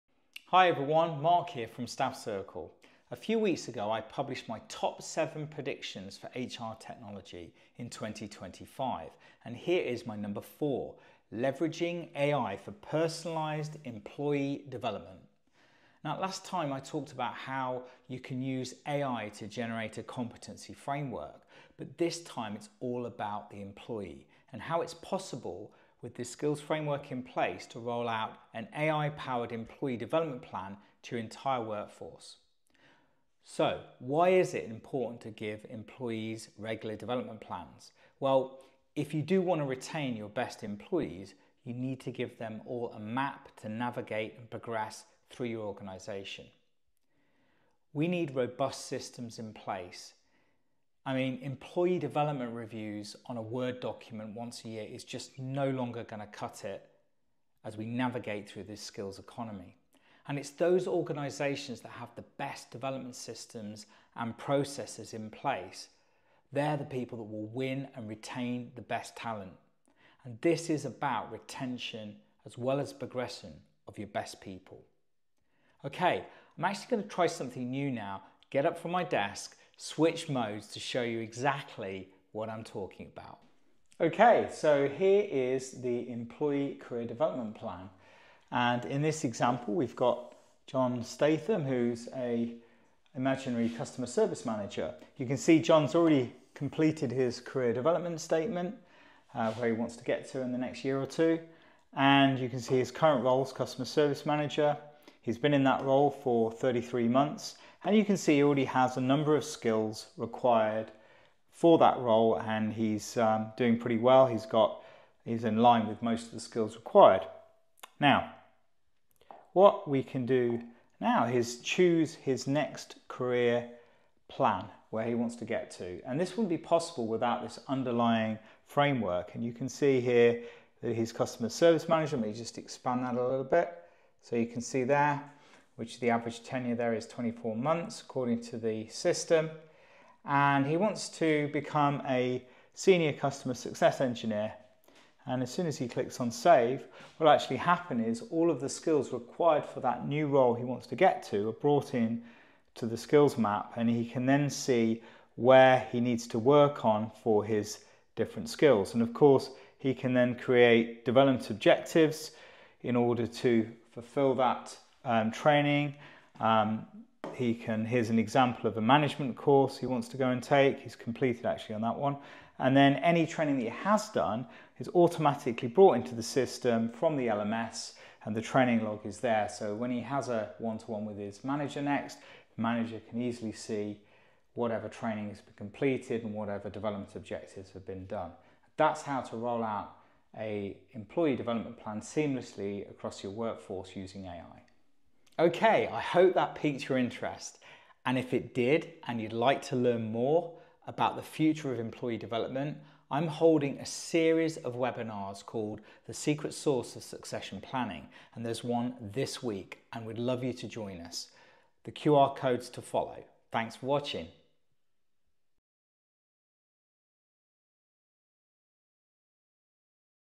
7 HR Tech Predictions for 2025 No 4 - Leveraging AI for Personalised Employee Development
7 HR Tech Predictions for 2025   No 4 - Leveraging AI for Personalised Employee Development - I discuss and demo how AI can be integrated into personal development plans helping to reduce attrition in your workforce.